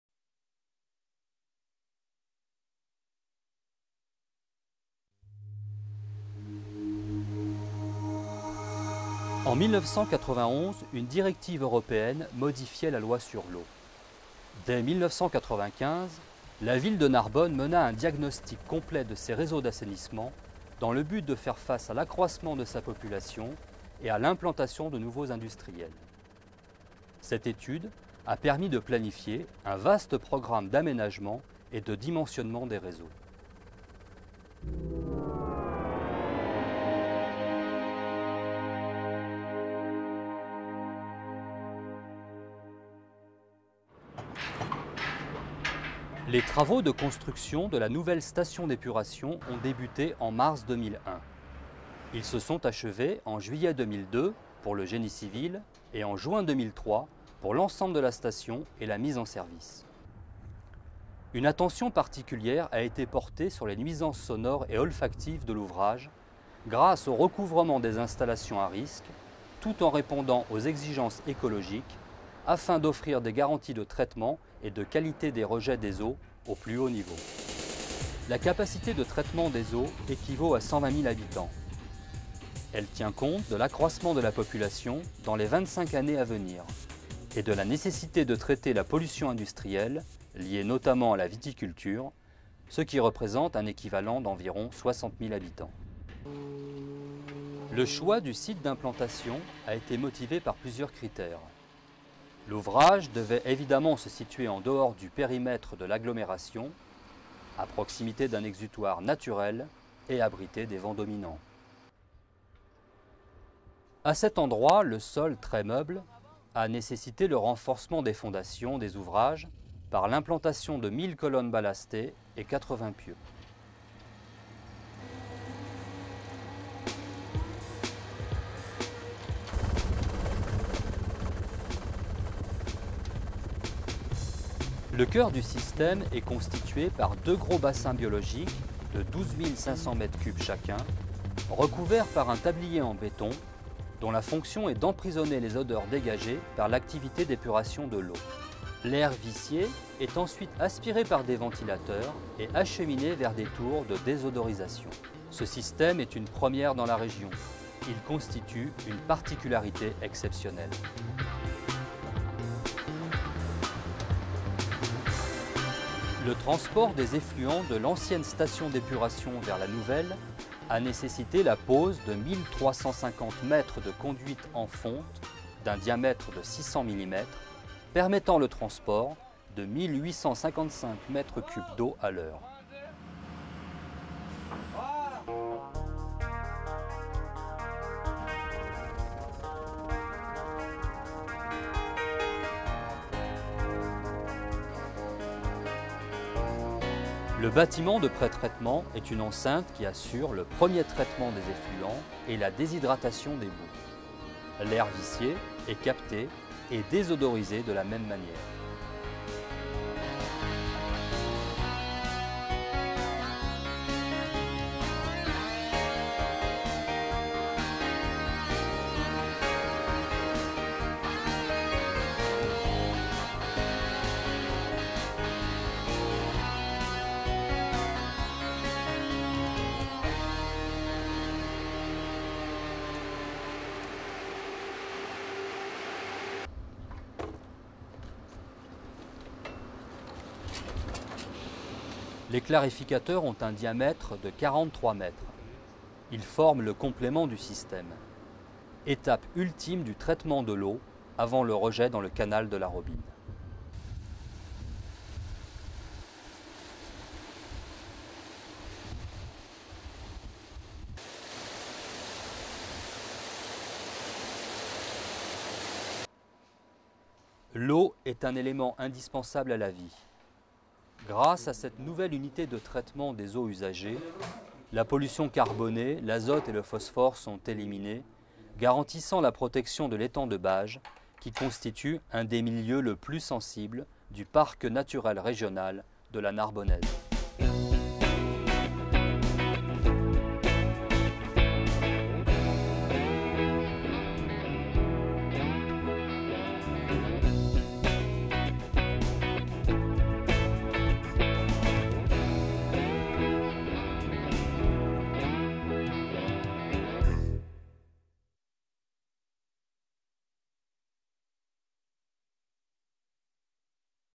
GENIE CIVIL : Station d'épuration de Narbonne
La construction et le fonctionnement de la station d'épuration des eaux de Narbonne.